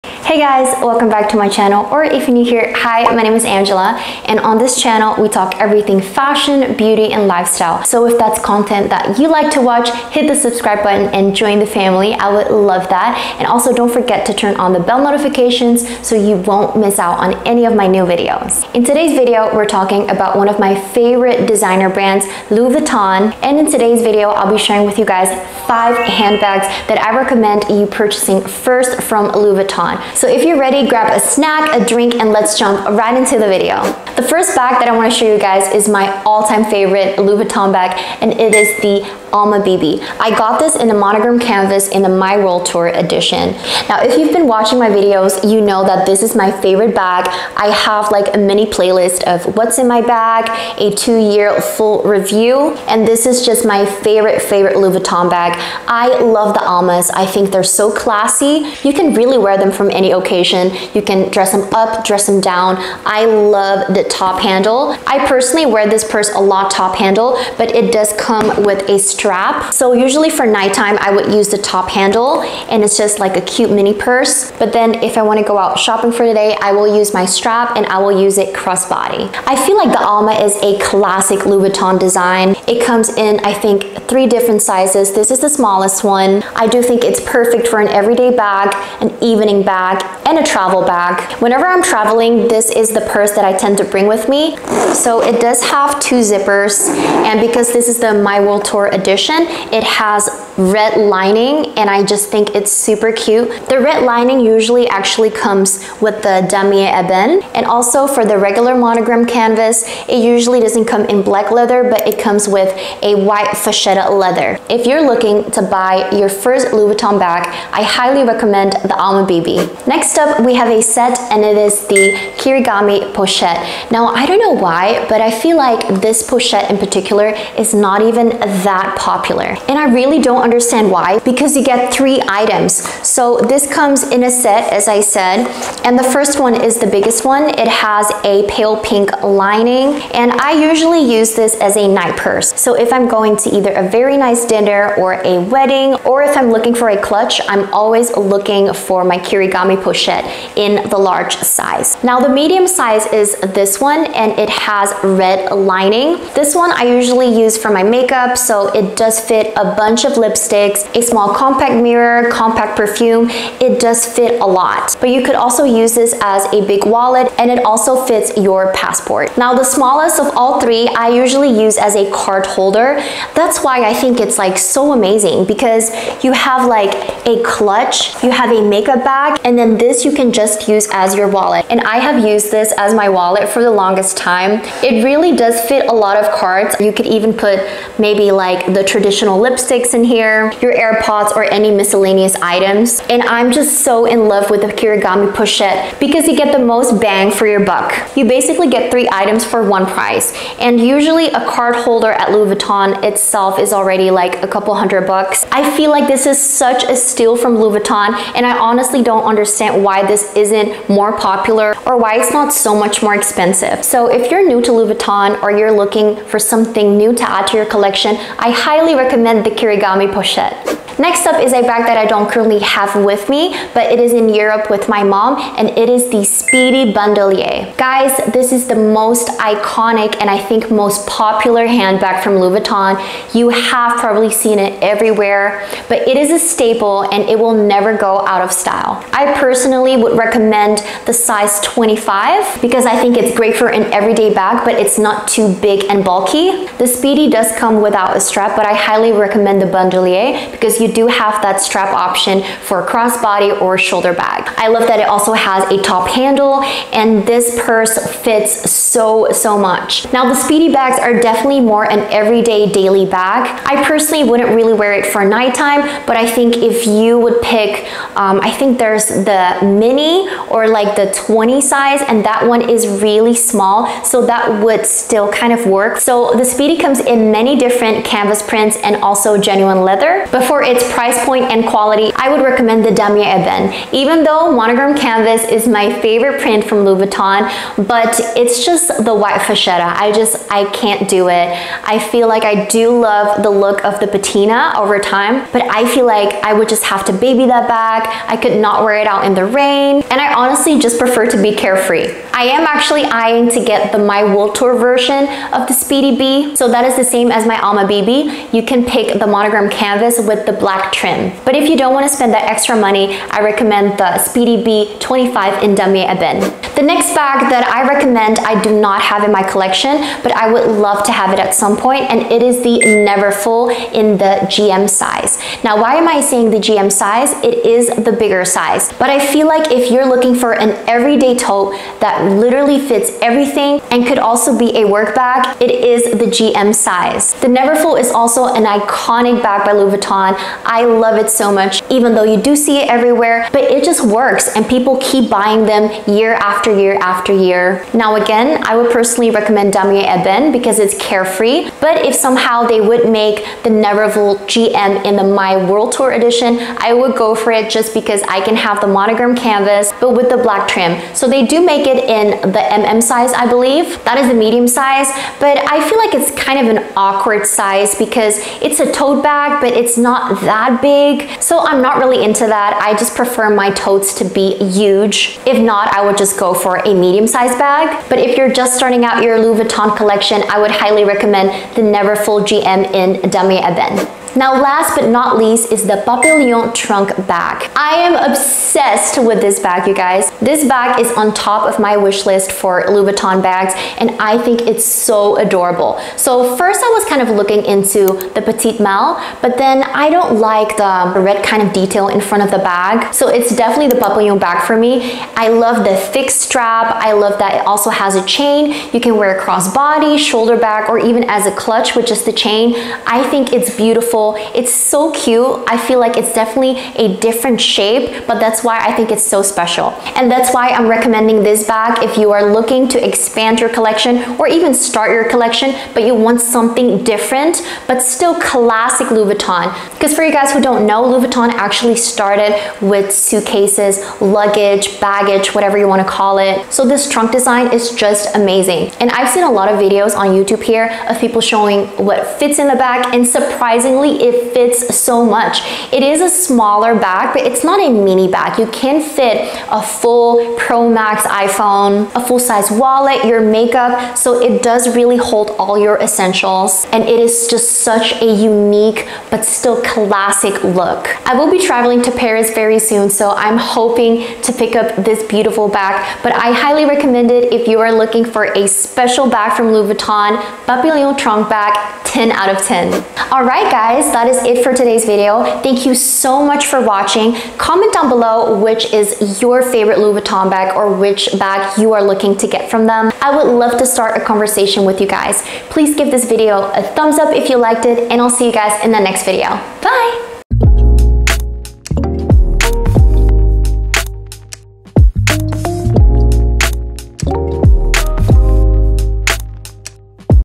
Best First Louis Vuitton Bags You Must Have
Don’t forget to “like” this video if it was helpful.

If you choose to purchase after clicking a link,
Thanks for supporting my channel by using my links!

+ CHAPTERS
00:00 - Intro
00:34 - Alma BB
02:00 - Kirigami Pochette
03:41 - Speedy B 25
05:17 - Neverfull GM
06:24 - Papillon Trunk
07:53 - Closing